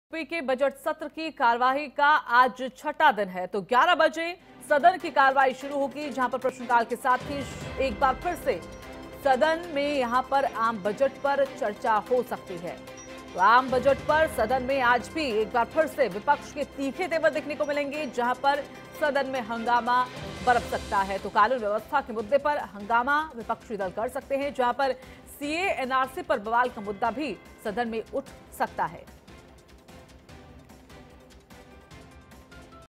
UP Budget2020 : यूपी के बजट सत्र की कार्यवाही का छठा दिन , 11 बजे होगी सदन की कार्यवाही R9TV
प्रश्नकाल के साथ शुरू होगी सदन की कार्यवाही
आम बजट पर सदन में आज होगी चर्चा